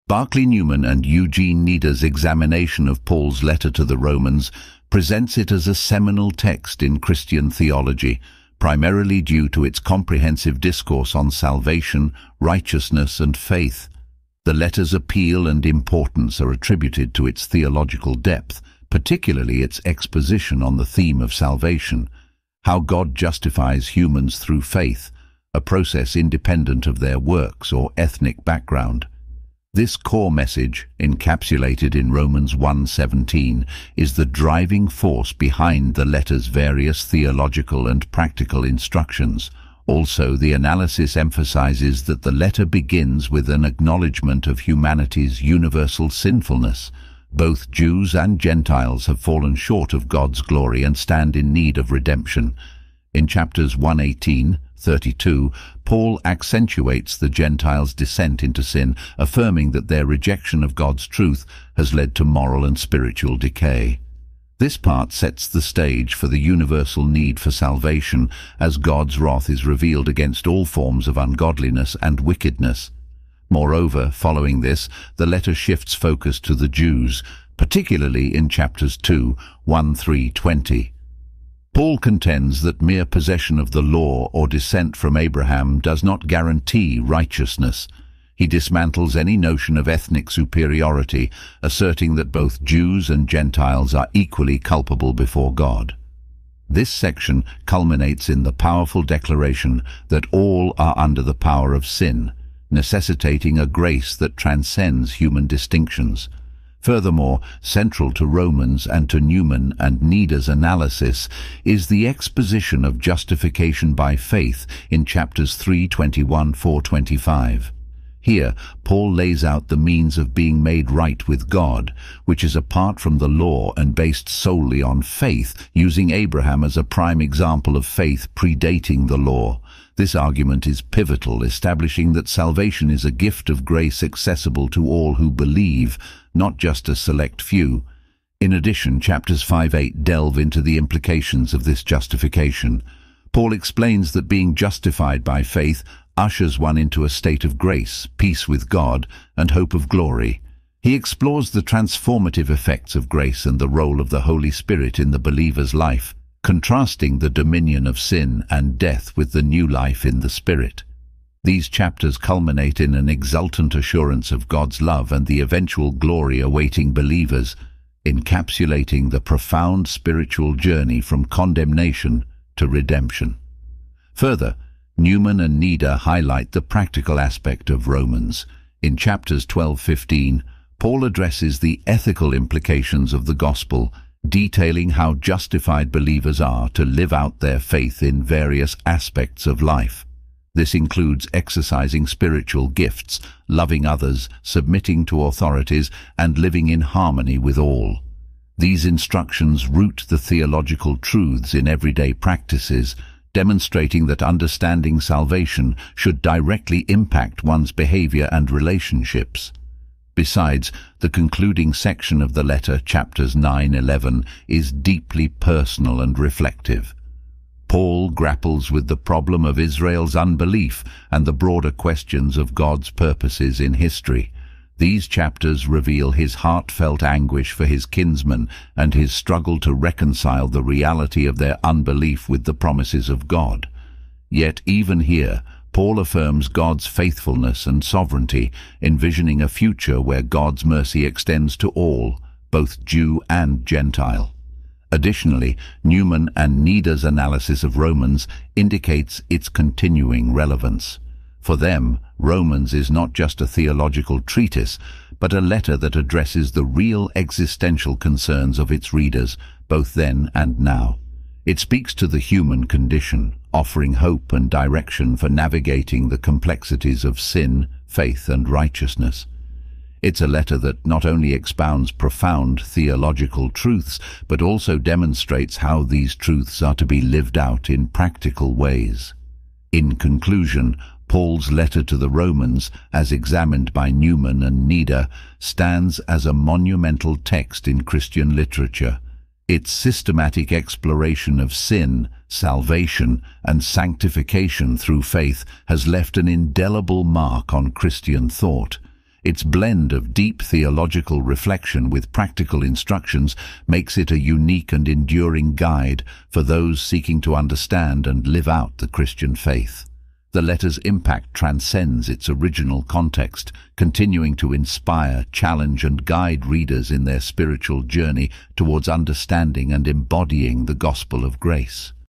A Handbook on Paul’s Letter to the Romans by Barclay M. Newman and Eugene A. Nida - Romans Introd...
Barclay Newman and Eugene Nida's study of Paul's Letter to the Romans highlights its significance as a foundational text in Christian theology, offering an in-depth discourse on salvation, righteousness, and faith. The letter is celebrated for its theological depth, particularly its exposition on salvation — a divine act of justifying humans through faith, irrespective of their deeds or ethnic origins, as highlighted in Romans 1:17. The letter starts by acknowledging the universal sinfulness of humans, setting a stage for the need for salvation against God's wrath due to ungodliness and wickedness.

Paul emphasizes that neither Jews nor Gentiles are exempt from sin, dismissing any claim of ethnic superiority. The crucial concept of justification by faith is elaborated, citing Abraham's example to illustrate that righteousness comes from faith, not law, making salvation accessible to all. Chapters 5-8 discuss the implications of justification, describing a transformative grace that brings peace with God and a new life through the Spirit. Paul assures believers of God's love and the glory that awaits them, moving from condemnation to redemption.

Practical aspects of living out this faith are detailed in chapters 12-15, where Paul advises on ethical living, exercising spiritual gifts, and harmonious living. The letter also addresses the issue of Israel's unbelief and God's overarching plan, reflecting Paul's anguish and hope for a future where God's mercy is extended to all.

Newman and Nida underscore the letter's ongoing relevance, viewing it not merely as a theological text but as a guide addressing existential human concerns. It offers hope and direction for sin, faith, and righteousness, emphasizing how these theological concepts should influence practical living. Ultimately, Paul's Letter to the Romans is portrayed as a monumental work, profoundly impacting Christian thought and practice, guiding believers in their spiritual journey through its intricate blend of theological depth and practical wisdom.

This summary is made by Eleven Labs AI audio generated platform